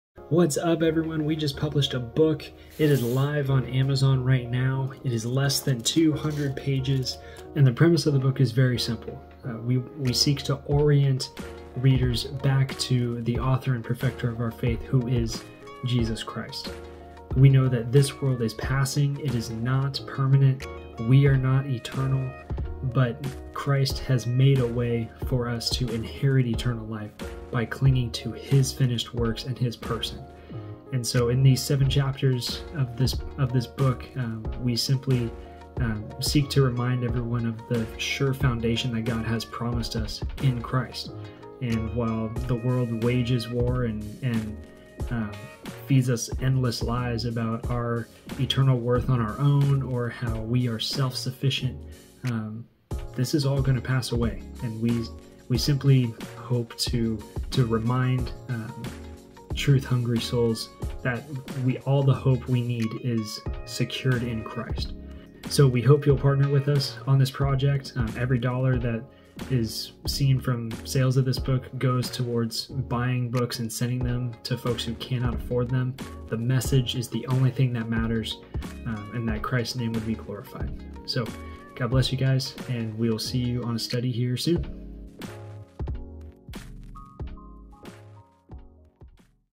BOOK RELEASE: Christ Is Reality!!
Our first publication is live here:

Necessary orientation and grounding for following Christ in these times.

After a year's worth of work (and Amazon almost blocking us) we are excited to get this out!